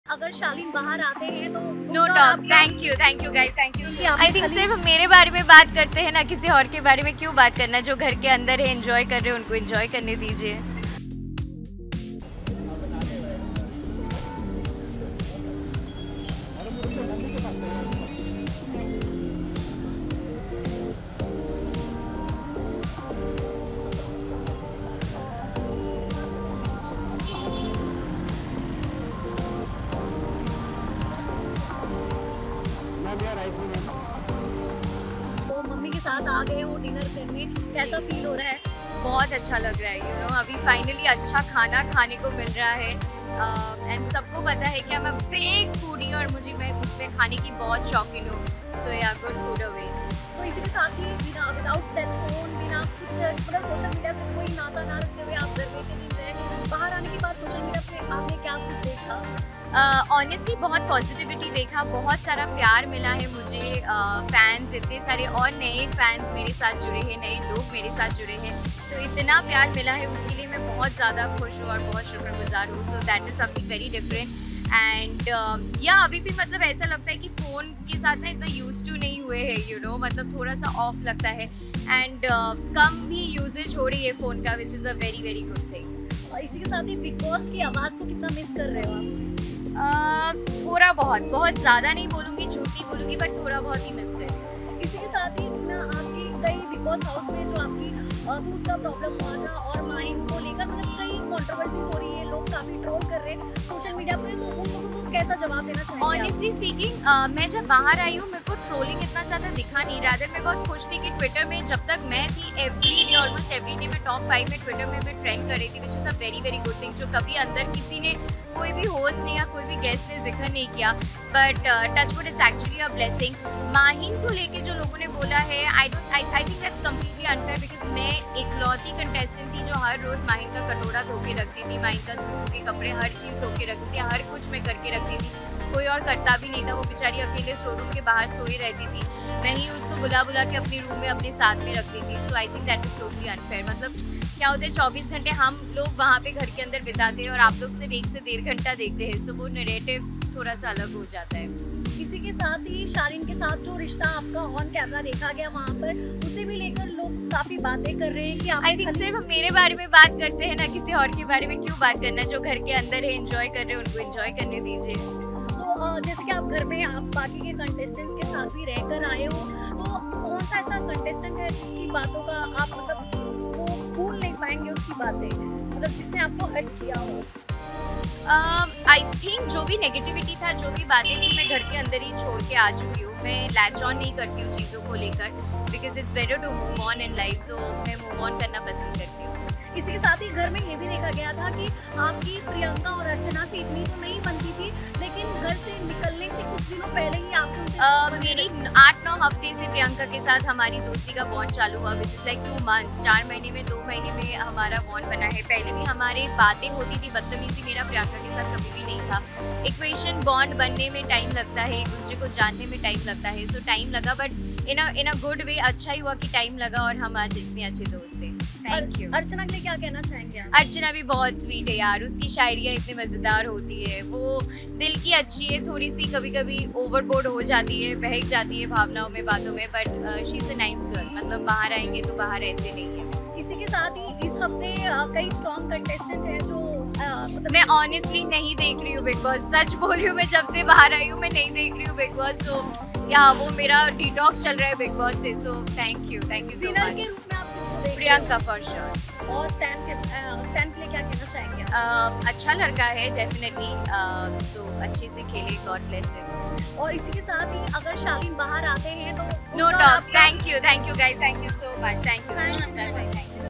Bigg Boss 16: Tina Dutta ने,Shalin, Mc Stan, Shiv, Priyanka के बारे में की बात, बताया अपना Winner!
Bigg Boss 16: Tina Dutta Dinner Date with her Mom, talks about Trolls & BB16 winner. Tina Dutta has come outside the BB house. Recently, she was spotted with her mother. Tina Dutta talks about Priyanka's game, Trolls, Mc Stan & Shalin. Tina ignores all the questions related to Shalin Bhanot. Watch all BB16 updates of FilmiBeat. Watch video to know more